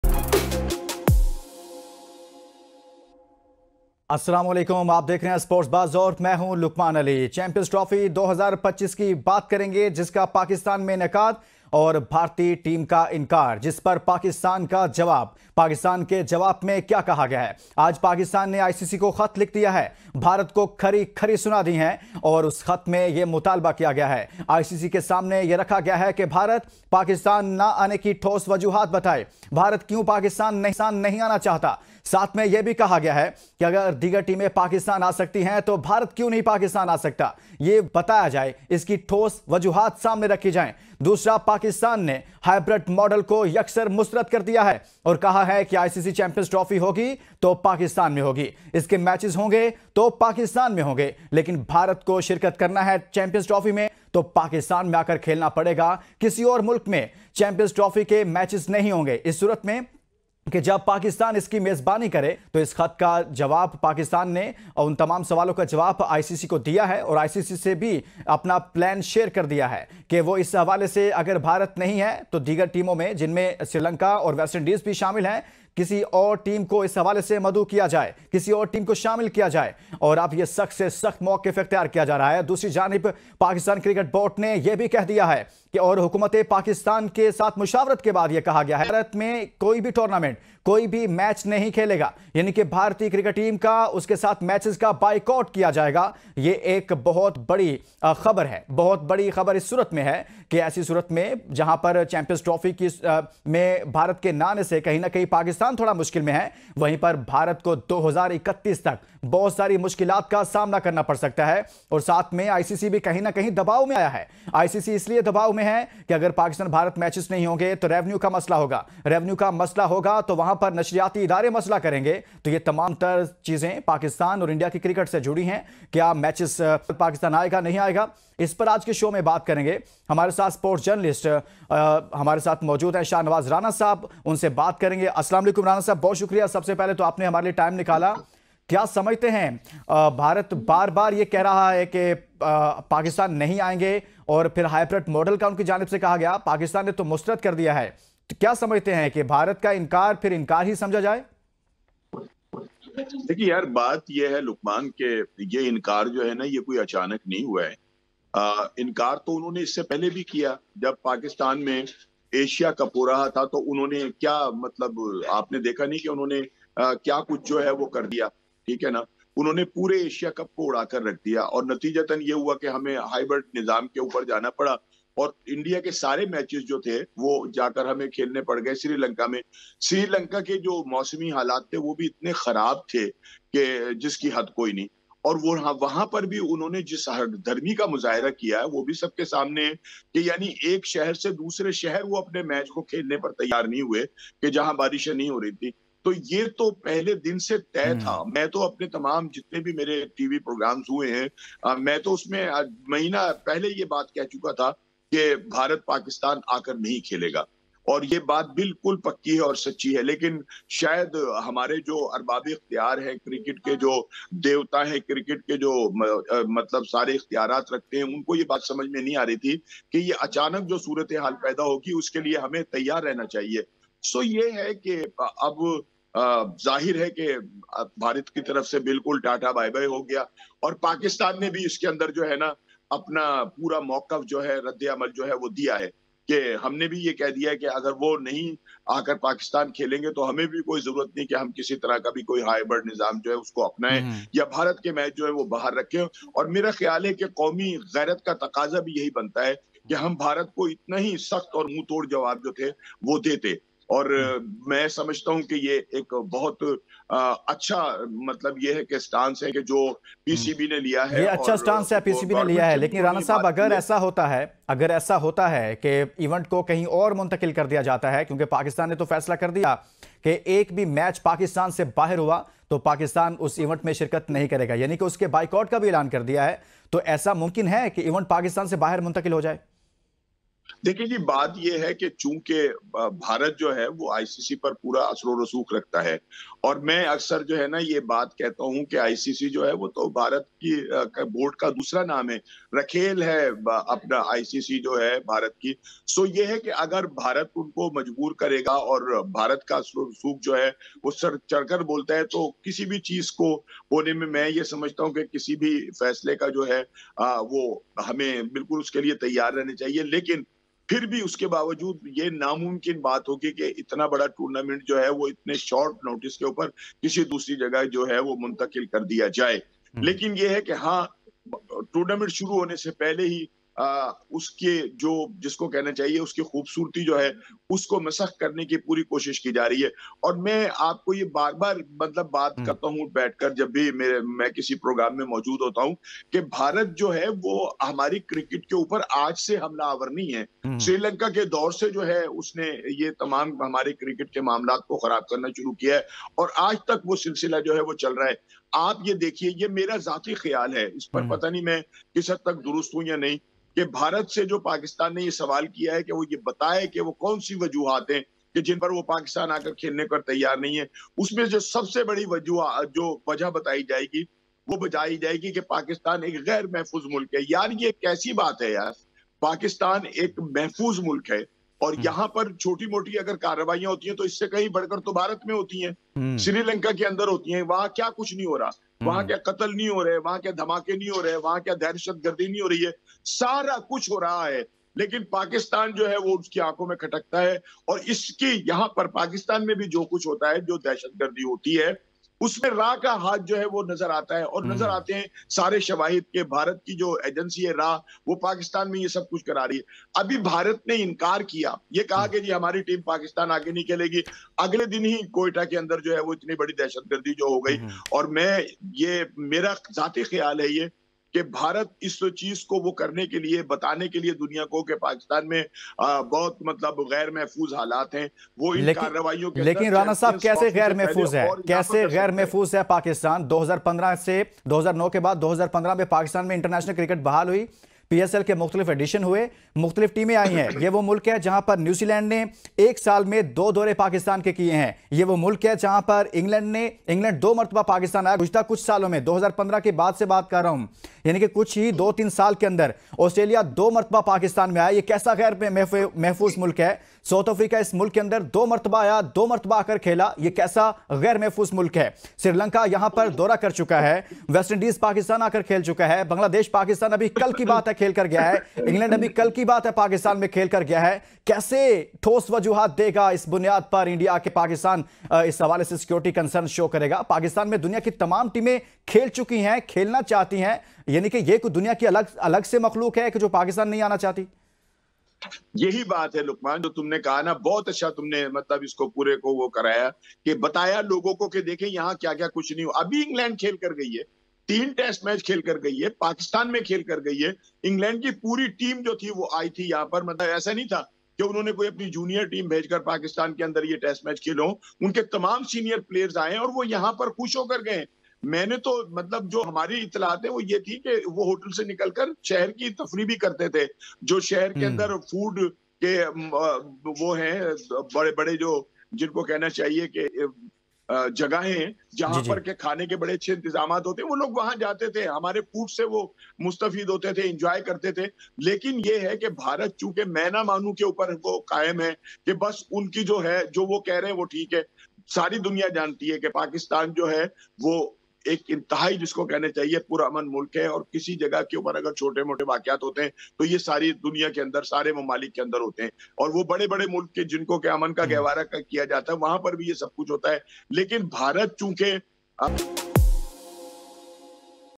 AGR BHARAT NA AYA TO HAM B NAHI JAYENGY, HYBERDMODEL B MANZOOR NAHI PAKISTAN KA ICC KA KHAAT
AGR BHARAT NA AYA TO HAM B NAHI JAYENGY, HYBERDMODEL B MANZOOR NAHI PAKISTAN KA ICC KA KHAAT SAKHAT MOQUF